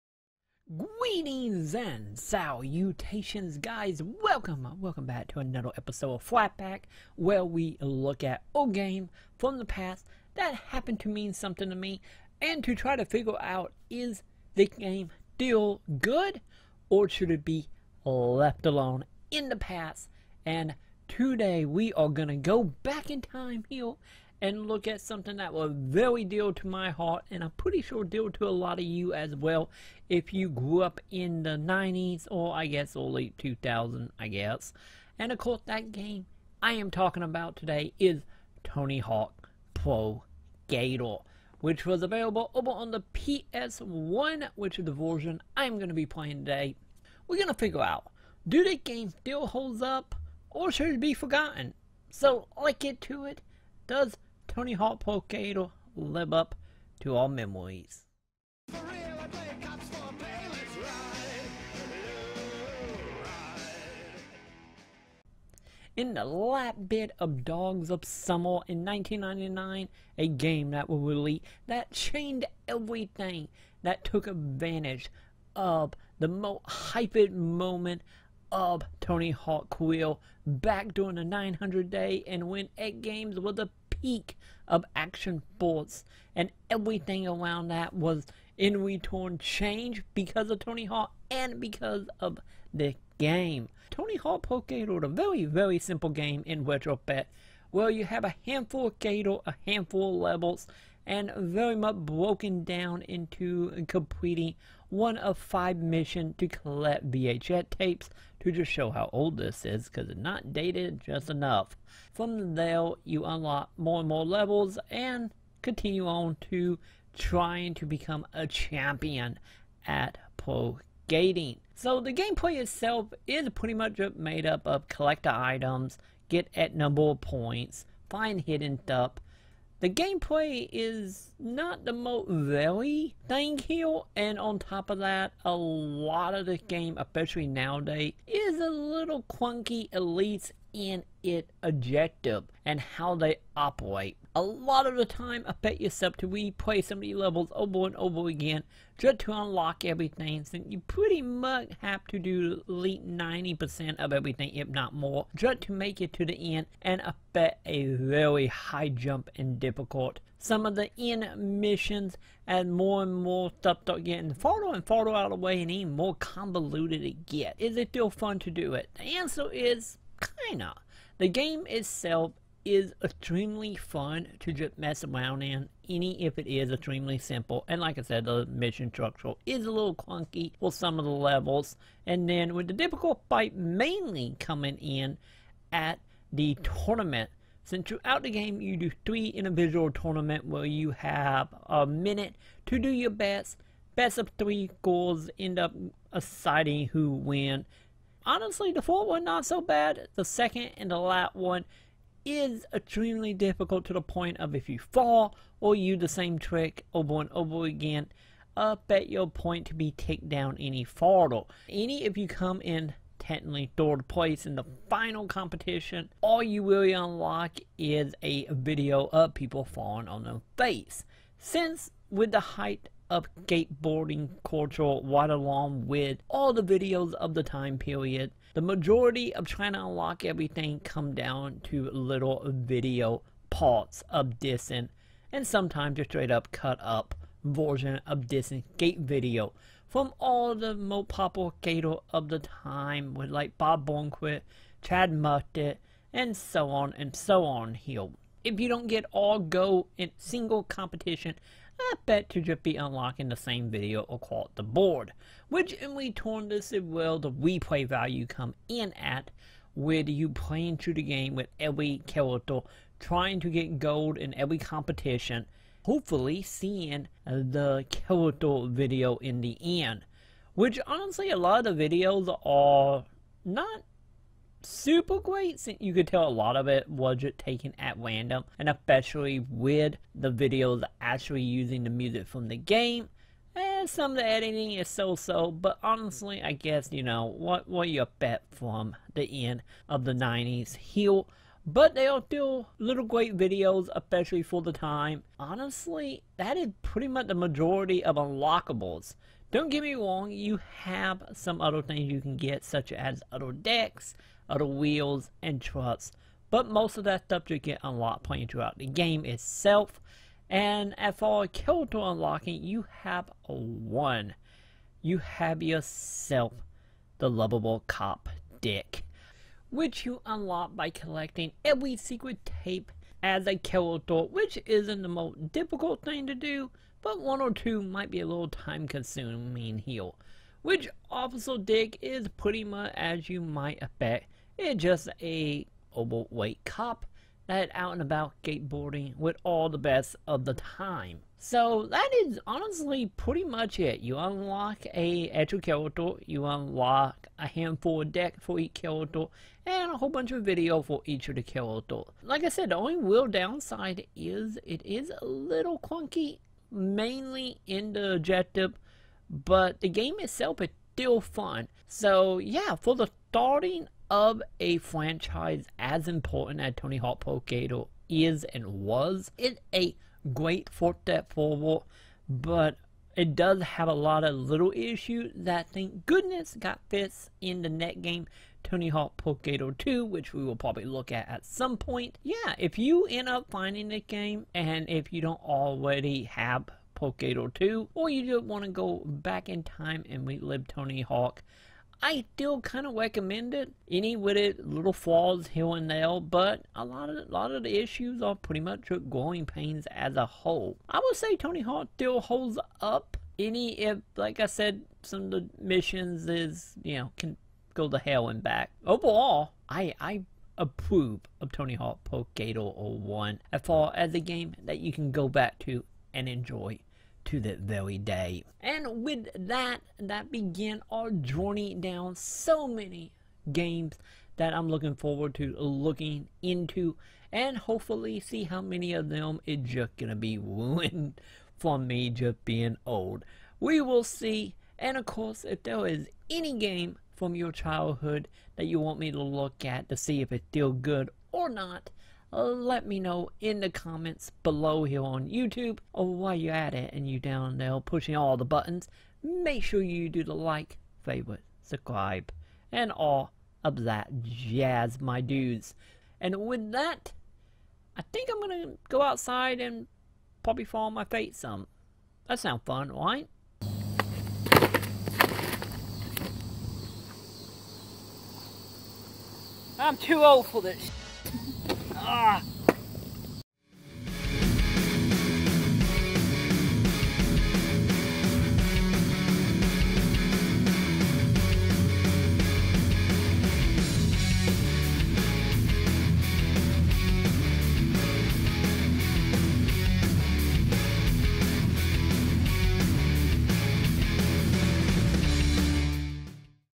Why Tony Hawks Pro Skater Is The Best Skateboarding Game: Retro Review
Let go back in time and see if Tony Hawk still got it in today review of Tony Hawks Pro Skater for the PlayStation 1. Is this game still one of the Legend of gaming or do you need to just go watch a skating video or jump on to a real skateboard.